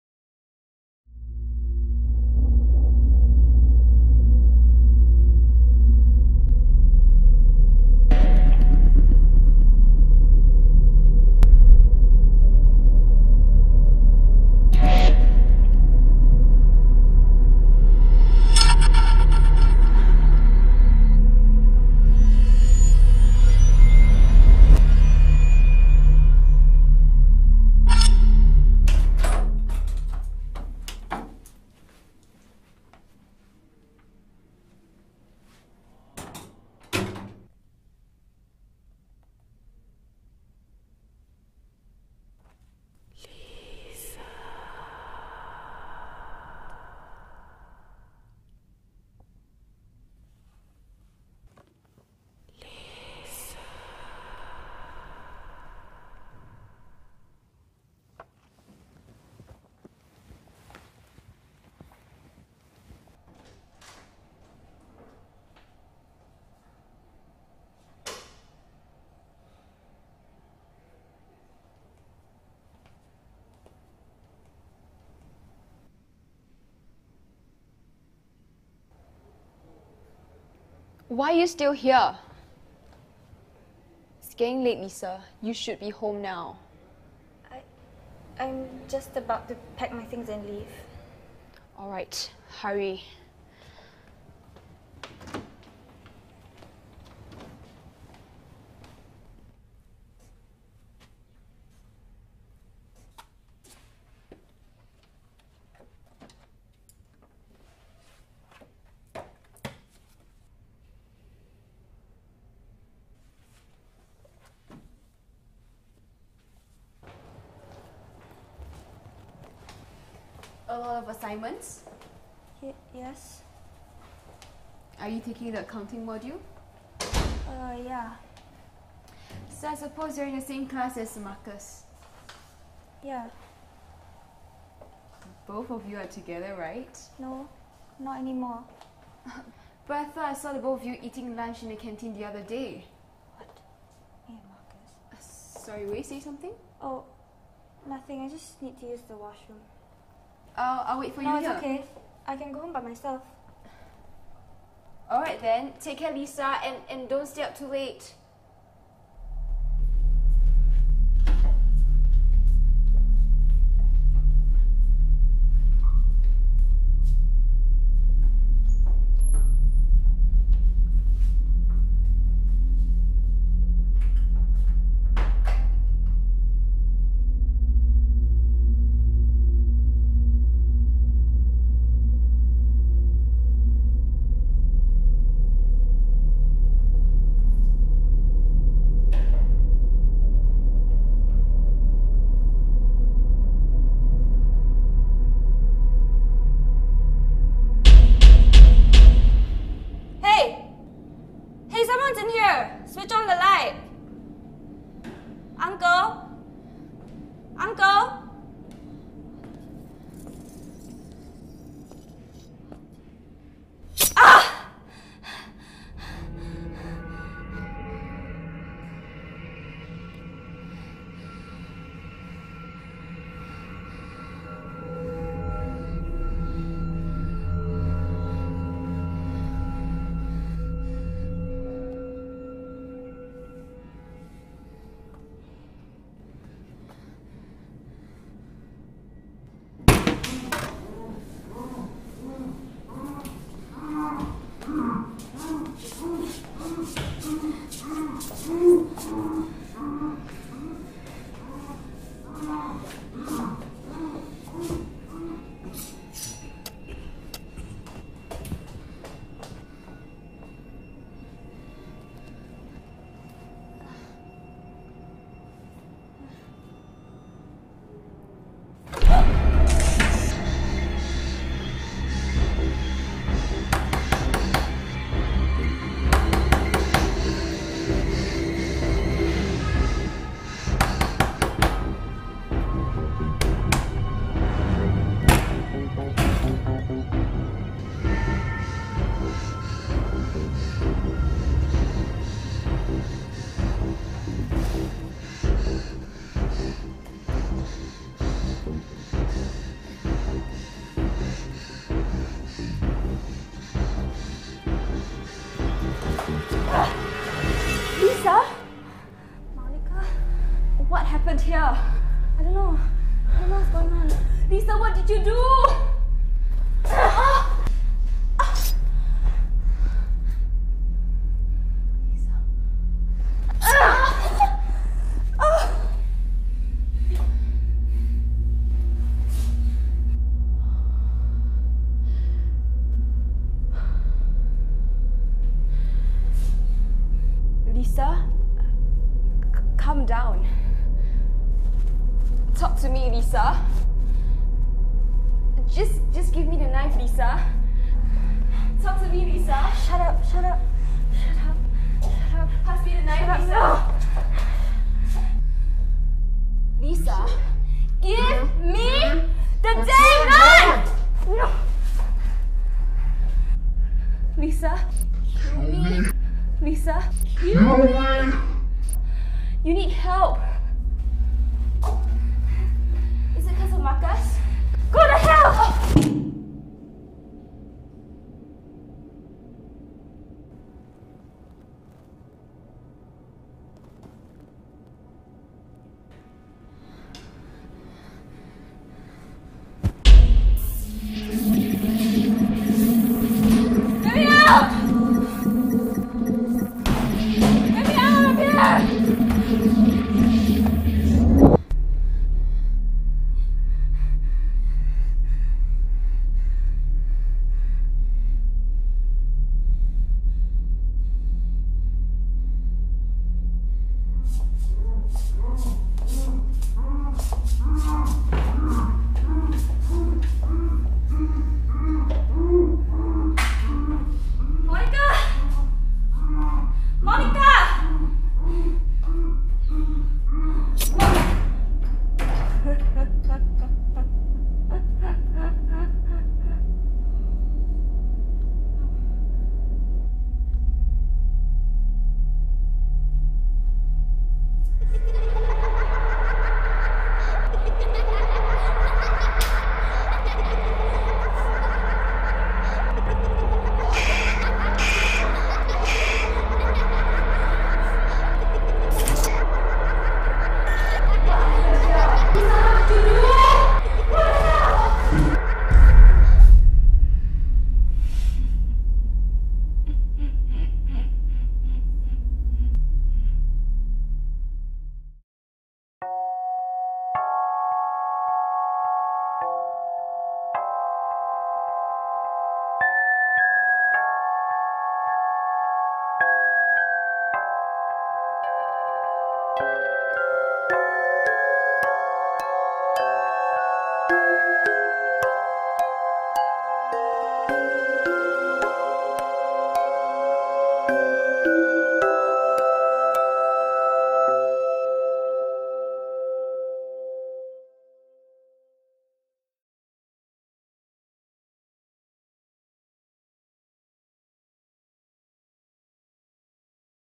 Underneath
School Assignments - Thriller Assigment

Synopsis
It's turning out to be a terrifying night for Lisa who is trapped in the toilet cubicle. A murder is happening right beside her as she tries to escape the hell around. Help arrives in the form of her lecturer but Lisa's own will bring her to brink of sanity